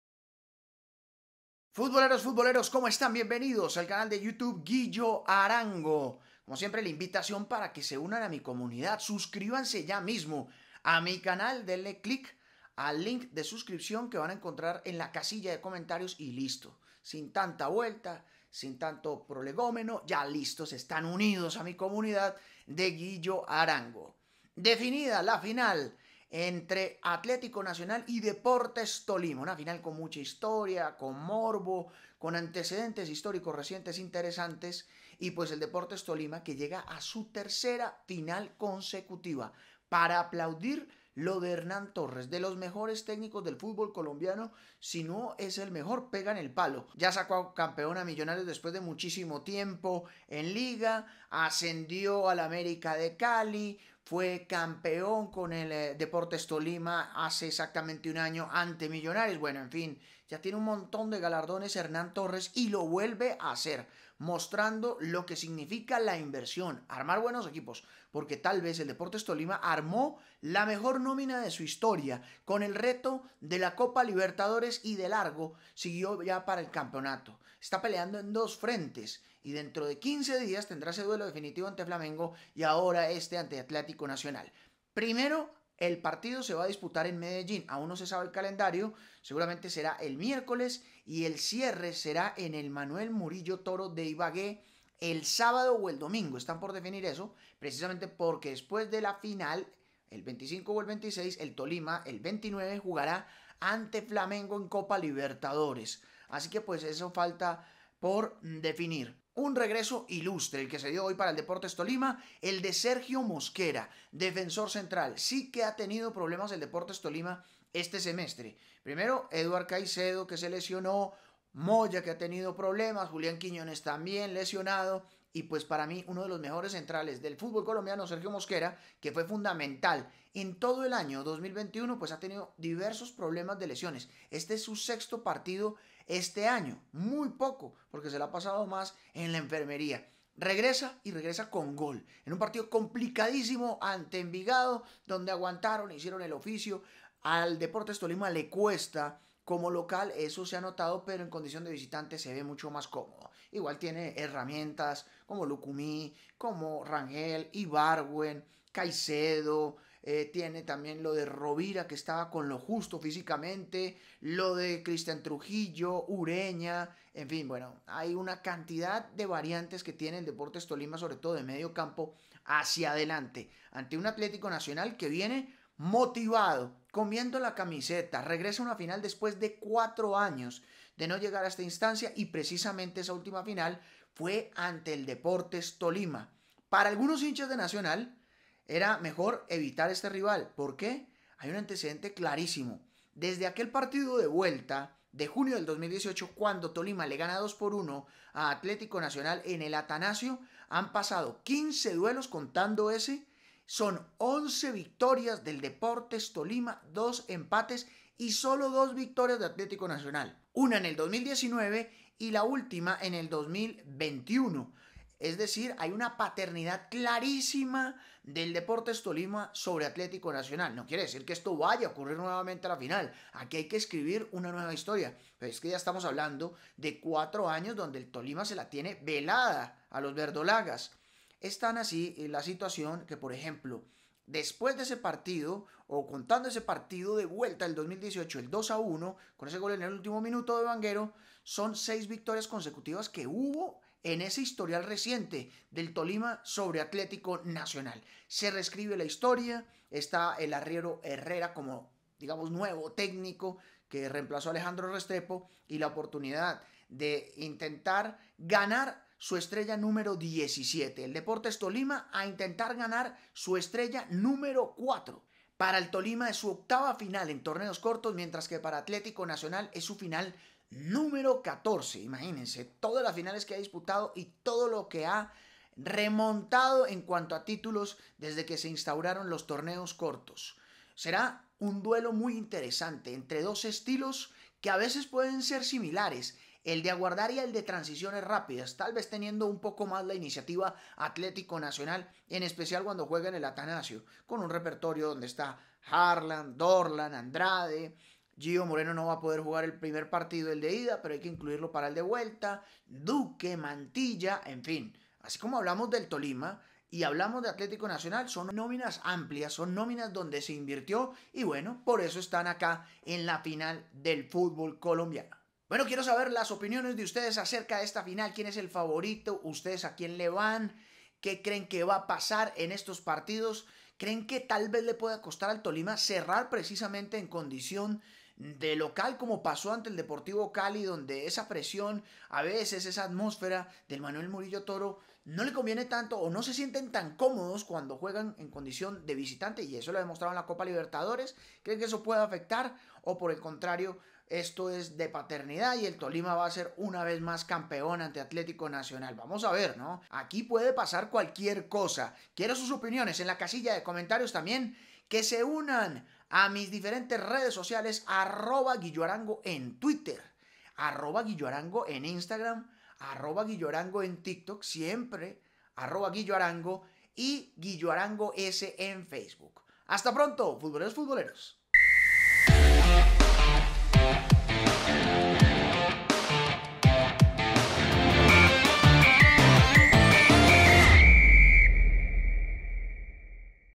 🏆 LA FINAL: ATLÉTICO NACIONAL VS DEPORTES TOLIMA
🖥 Me pueden seguir en @guilloarango en Twitter, Instagram y Tik Tok.

Definida la final de la Liga Betplay, Tolima vs Atlético Nacional.